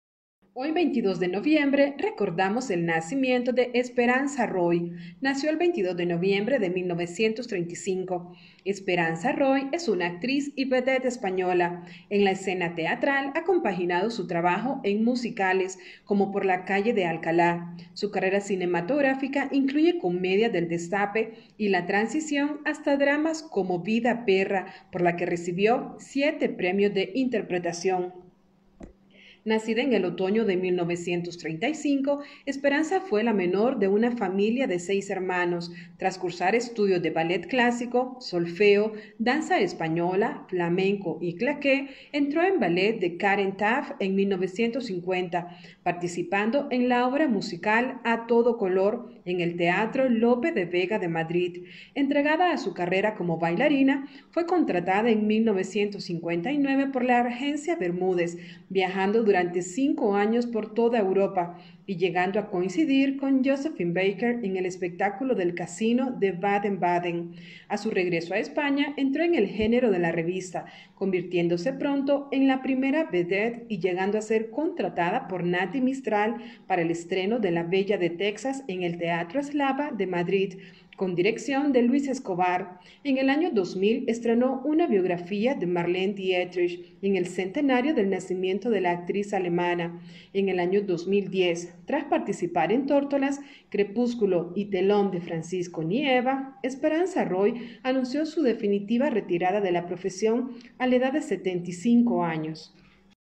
ESPERANZA ROY
Nacimiento: 22 de noviembre de 1935

Biografía Es una actriz y vedette española. En la escena teatral, ha compaginado su trabajo en musicales como Por la calle de Alcalá. Su carrera cinematográfica, incluye comedias del "destape" y la Transición hasta dramas como Vida/Perra por la que recibió siete premios de interpretación. Nacida en el otoño de 1935, Esperanza fue la menor de una familia de seis hermanos. Tras cursar estudios de ballet clásico, solfeo, danza española, flamenco y claqué, entró en el ballet de Karen Taft en 1950, participando en la obra musical A todo color, en el teatro Lope de Vega de Madrid.

Entregada a su carrera como bailarina, fue contratada en 1959 por la Agencia Bermúdez viajando durante cinco años por toda Europa, y llegando a coincidir con Josephine Baker en el espectáculo del casino de Baden-Baden. A su regreso a España entró en el género de la revista, convirtiéndose pronto en primera «vedette», y llegando a ser contratada por Nati Mistral para el estreno de La bella de Texas en el Teatro Eslava de Madrid, con dirección de Luis Escobar.

En 2000 estrenó una biografía de Marlene Dietrich en el centenario del nacimiento de la actriz alemana. En 2010, tras participar en Tórtolas, crepúsculo y telón de Francisco Nieva, Esperanza Roy anunció su definitiva retirada de la profesión a la edad de 75 años.

Biography She is a Spanish actress and vedette. In the theater scene, she has combined her work in musicals such as Por la calle de Alcalá. Her film career includes comedies from "uncover" and Transition to dramas like Life / Perra for which she received seven acting awards. Born in the fall of 1935, Esperanza was the youngest in a family of six siblings. After studying classical ballet, music theory, Spanish dance, flamenco and tap, she entered Karen Taft's ballet in 1950, participating in the musical A todo color, at the Lope de Vega theater in Madrid.

She dedicated to her career as a dancer, she was hired in 1959 by the Bermúdez Agency, traveling for five years throughout Europe, and eventually meeting Josephine Baker at the Baden-Baden casino show. Upon her return to Spain, she entered the magazine genre, soon becoming the first "star", and eventually being hired by Nati Mistral for the premiere of La bella de Texas at the Eslava Theater in Madrid, directed by Luis Escobar. In 2000 she released a biography of Marlene Dietrich on the centenary of the German actress's birth. In 2010, after participating in Tórtolas, twilight and curtain by Francisco Nieva, Esperanza Roy announced her final retirement from the profession at the age of 75.

Filmography
• Dos chicas locas, locas (1964), de Pedro Lazaga. • ¿Qué hacemos con los hijos? (1967), de Pedro Lazaga. • Si volvemos a vernos (1968), de Francisco Regueiro. • Pecados conyugales (1969), de José María Forqué. • ¿Por qué pecamos a los cuarenta? (1969), de Pedro Lazaga. • El cronicón (1969), de Antonio Giménez Rico. • El jardín de las delicias (1970), de Carlos Saura. • Guapo heredero busca esposa (1972), de Luis María Delgado. • Las señoritas de mala compañía (1973), de José Antonio Nieves Conde. • Un casto varón español (1973), de Jaime de Armiñán. • El ataque de los muertos sin ojos (1973), de Amando de Ossorio. • Una vela para el diablo (1973), de Eugenio Martín. • Dormir y ligar: todo es empezar (1974), de Mariano Ozores. • El insólito embarazo de los Martínez (1974), de Javier Aguirre. • Es pecado... pero me gusta (1977), de Juan Bosch. • Gusanos de seda (1976), de Francisco Rodríguez. • Memorias de Leticia Valle (1976), de Miguel Ángel Rivas. • El sacerdote (1978), de Eloy de la Iglesia. • Carne apaleada (1978), de Javier Aguirre. • ¡Tú estás loco, Briones! (1981), de Javier Maqua. • Vida/Perra (1982), de Javier Aguirre.17 • La monja alférez (1986), de Javier Aguirre. • Divinas palabras (1987), de José Luis García Sánchez. • Perdona bonita, pero Lucas me quería a mí (1997)

Créditos tomados de Wikipedia: